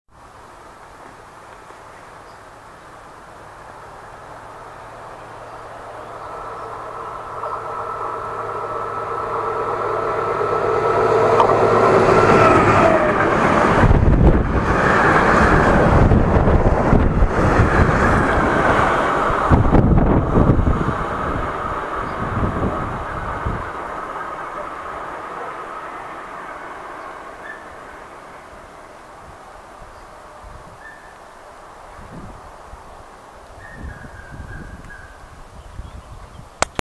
Unipetrol (Orlen Group) - Bizon 753.611-3 | Polanka nad Odrou - 6. 7. 2022 | 4k 24fps
Dneska jsem si pro vás připravil pouze tento krátký záběr z Polanky nad Odrou, konkrétně s Vectronem od firmy Unipetrol. Je to po velmi dlouhé době, co jsem takhle natočil průjezd vlaku. Kvalita videa je pouze experimentální.